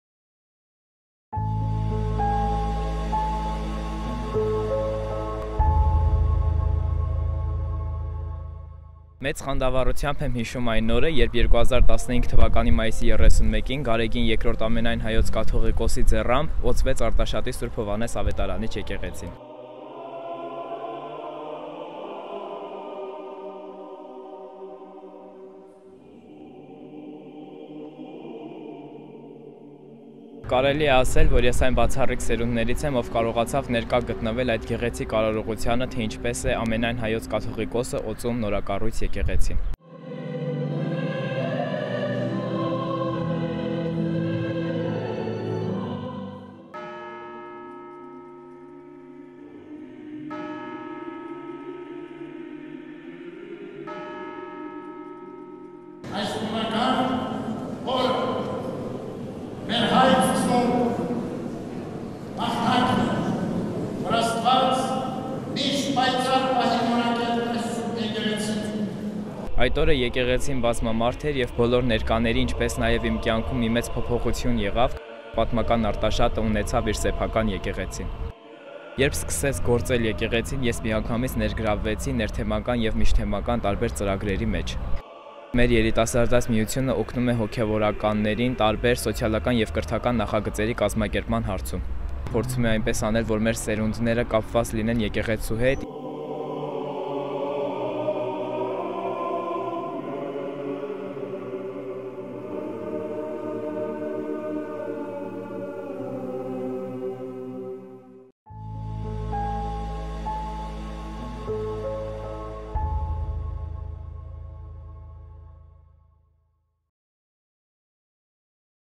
Արտաշատի Ս․ Հովհաննես Ավետարանիչ եկեղեցին ♰
ԻՄ ԵԿԵՂԵՑԻՆ, ԻՄ ՀԱՄԱՅՆՔԸ /
Սուրբ Հովհաննես Ավետարանիչ եկեղեցի, Արարատյան Հայրապետական թեմ, Արարատի մարզ, քաղաք Արտաշատ, 2015թ. ♰⤵️
Արտաշատ քաղաքի Ս. Հովհաննես Ավետարանիչ եկեղեցին, որի հիմնարկեքը կատարվել է 2002 թ. հոկտեմբերի 29-ին։ Եկեղեցին օծվել է 2015թ․-ին Ձեռամբ Ն.Ս.Օ.Տ.Տ. Գարեգին Բ Ծայրագույն Պատրիարք և Ամենայն Հայոց Կաթողիկոսի:
«Իմ Եկեղեցին, իմ համայնքը» ծրագիրը համալրում է Վեհափառ Հայրապետի 20-ամյա գործունեությանը նվիրված նախաձեռնությունների շարքը։

Ծրագիրը հնարավորություն է ընձեռում երիտասարդներին աշխարհի տարբեր անկյուններից միմյանց պատմելու Հայ Առաքելական Սուրբ Եկեղեցու՝ իրենց թեմի ժառանգության՝ Հայկական վանքերի ու եկեղեցիների մասին։

Այս նախաձեռնության շրջանակում ՀԵՀԵՄ թեմական երիտասարդները տեսաձայնագրությամբ ներկայացնում են իրենց համայնքի եկեղեցիներն ու դրանց պատմությունը։